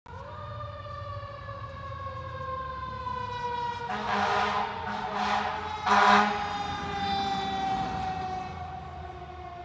Citizen's Hose Co.2, Seymour, CT Tower 14 responding to an appliance fire
Video from Andrew Eva

Tower 14 is a 2005 KME Aerialcat 2000/300/95' MMT